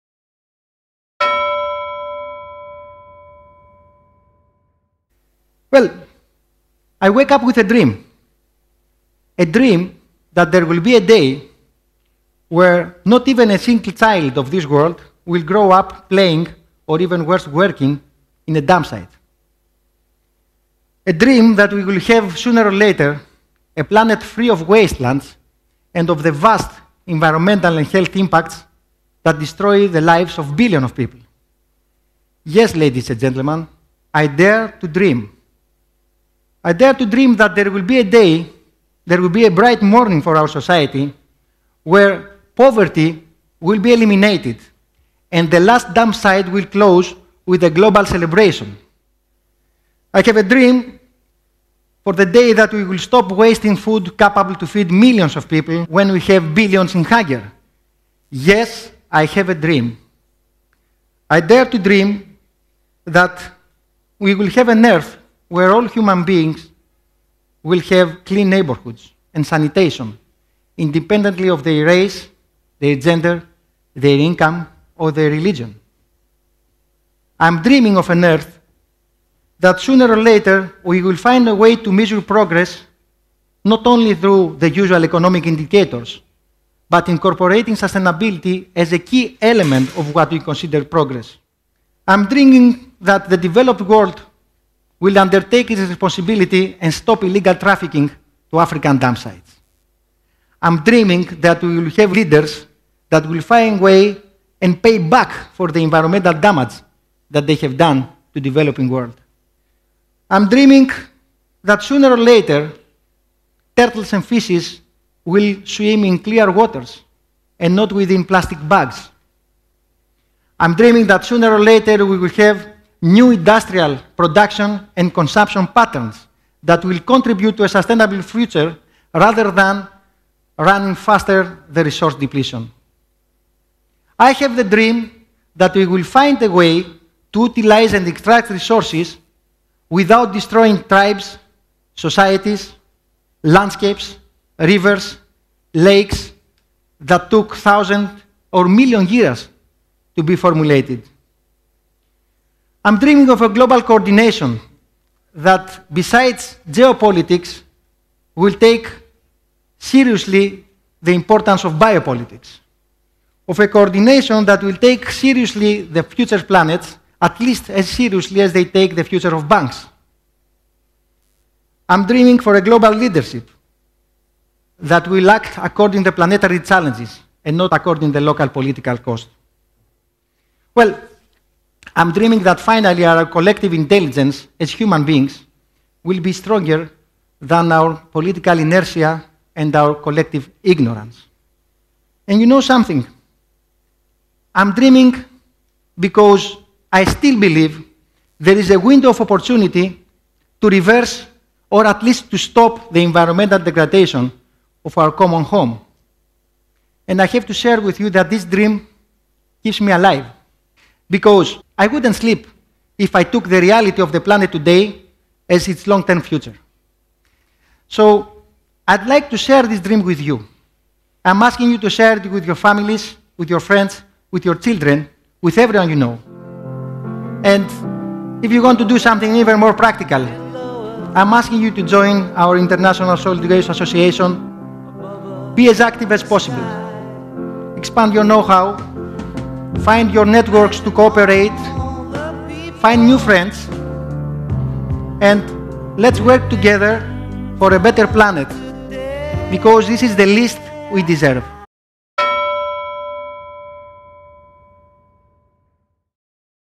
A Dream for a Wasteless Future
This was the closure of my lecture on the fourth industrial revolution and the possibilities created for the waste sector, in ISWA - ARS's conference in Buenos Aires, October 2015